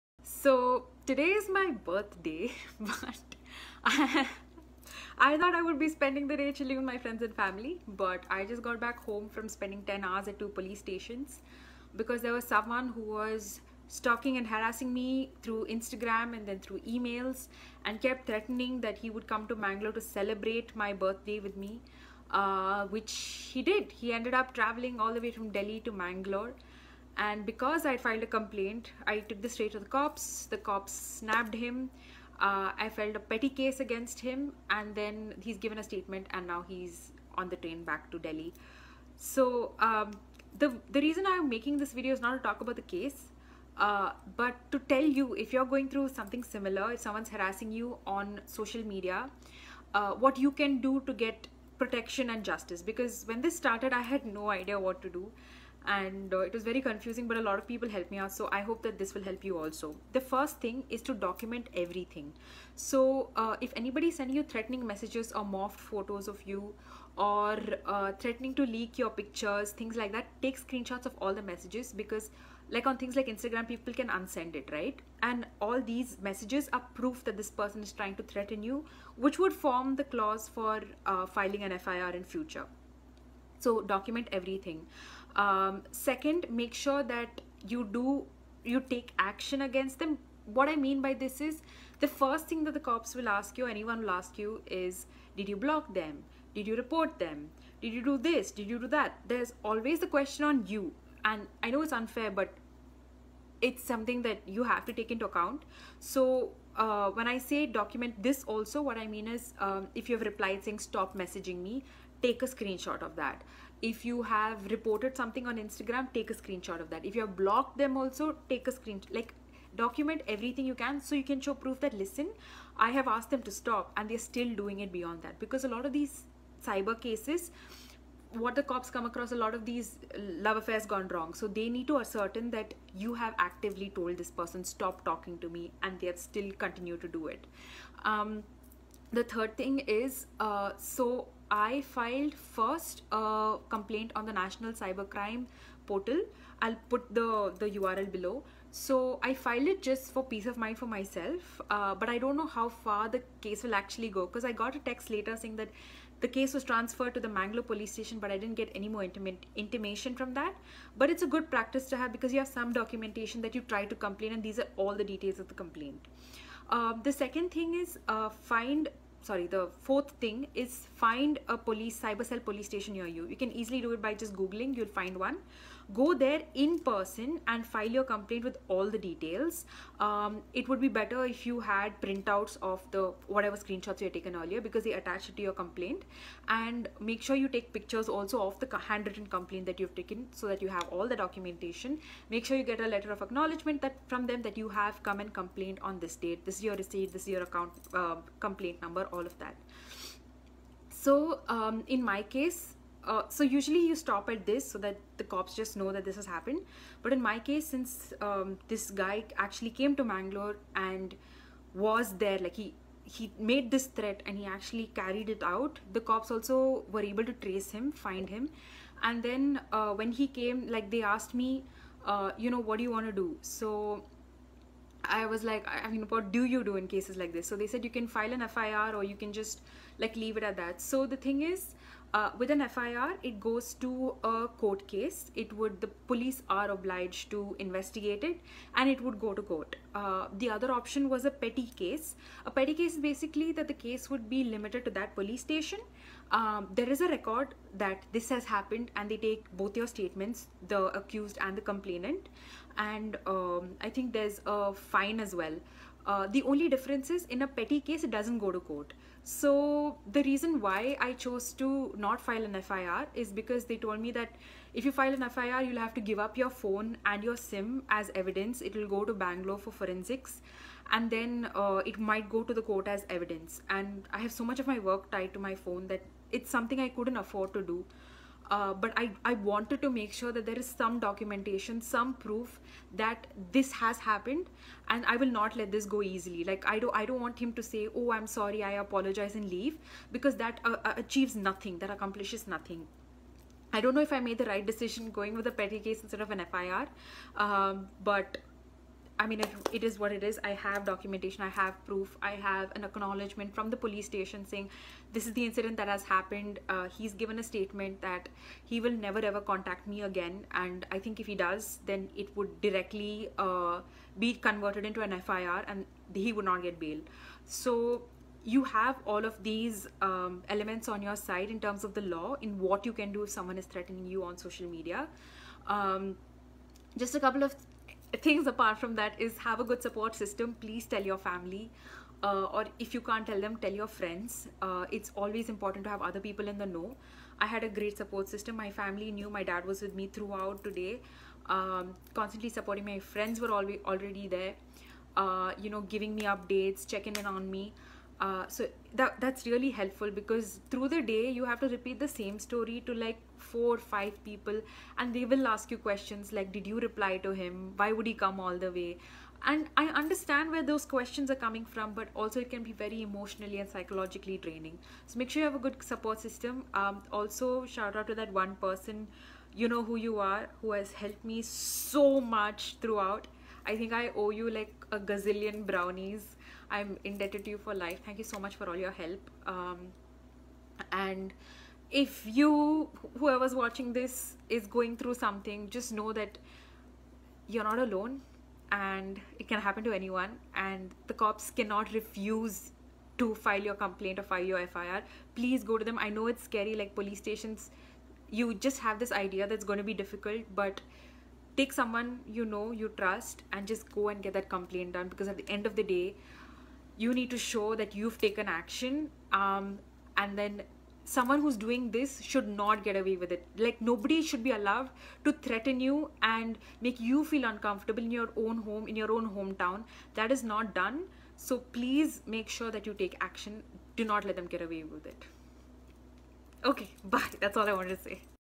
how to file a police complaint against harassment on social media in India | my experience
I had posted this on March 15, 2021 on my Instagram page - and since then have received a lot of messages, both saying that it really helped & also requesting to upload it here, as it's easier to access as compared to scrolling through my IG page.

I see you, I hear you and hence have shared it here as well. Here are the links mentioned in the video:

➡️Reach out to any cyber cell in your city - just google it, you will find the address. Go in person and file a complaint with the evidence.

*️⃣Thanks to the Cyber Cell and Women’s Police Station, Mangalore for your assistance.

⚠️Don’t let harassers get away, and please stop romanticising stalker behaviour. If someone says ‘stop texting me’ or blocks you, just let it go. If you keep bothering them then next person you meet will be a police officer.